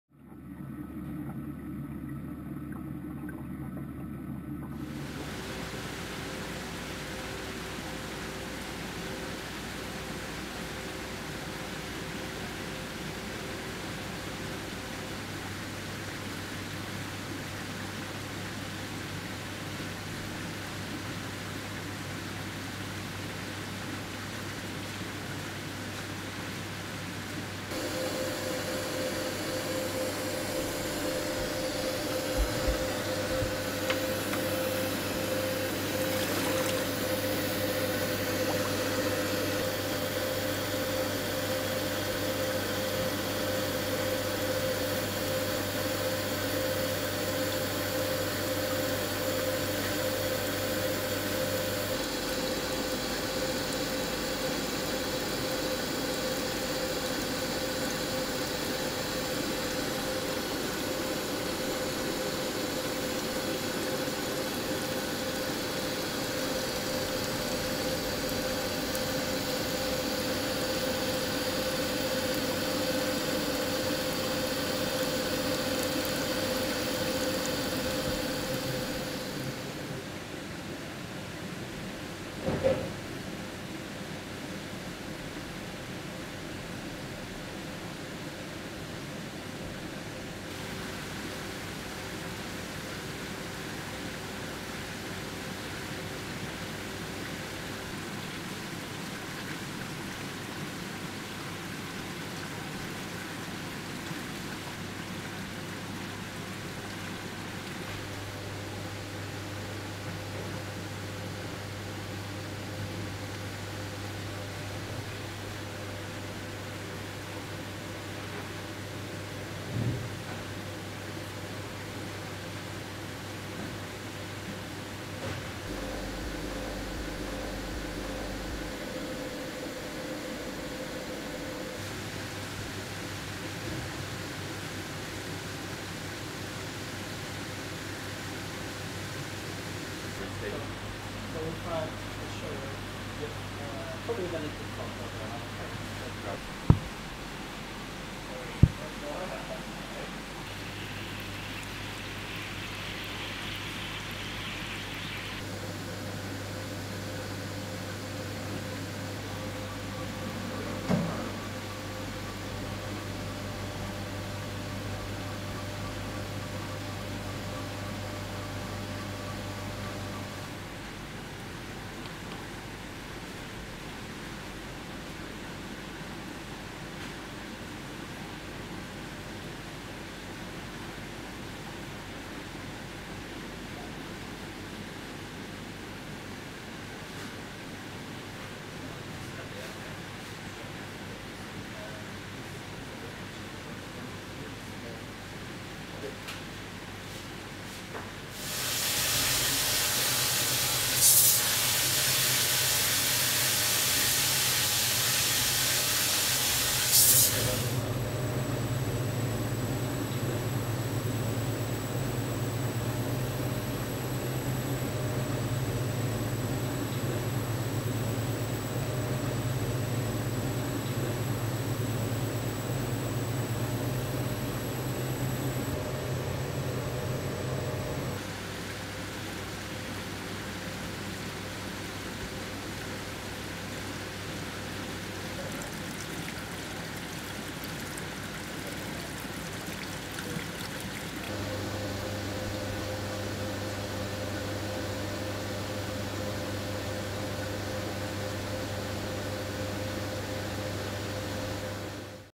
Breeding Zebrafish
This film shows some of the aspects of zebrafish production and maintenance. Zebrafish embryos, unlike mouse embryos, develop outside the mother's body and are transparent throughout the first few days of life. This provides unparallelled opportunities for researchers to scrutinise the fine details of embryonic vertebrate development without first having to resort to invasive procedures or killing the mother. This advantage is enhanced by the fact zebrafish can produce 200-300 fertilised eggs every week.
UAR can organise speakers to come to schools to explain their scientific work - see the website for details